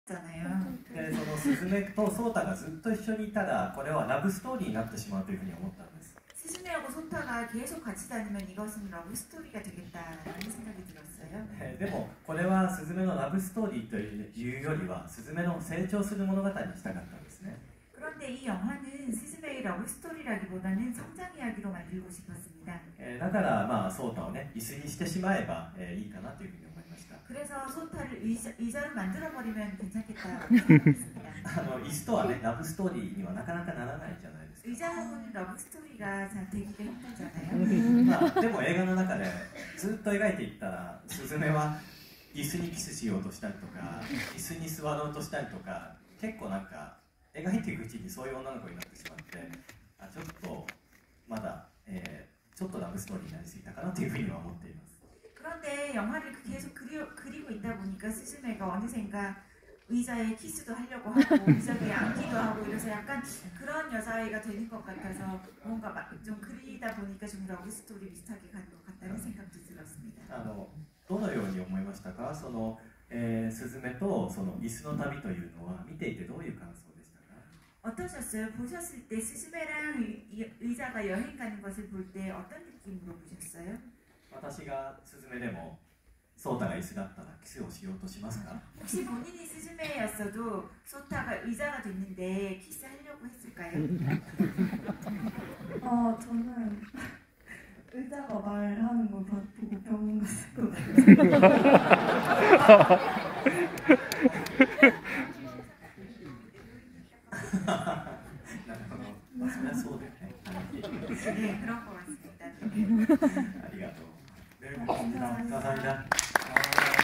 신카이 마코토 스즈메의 문단속 GV 영상
스즈메의 문단속 흥행감사 GV

500만이 되어 가는 스즈메의 문단속 흥행감사로 신카이 마코토 감독님이 내한 하셔서 한국판 성우분들과 GV를
가졌습니다.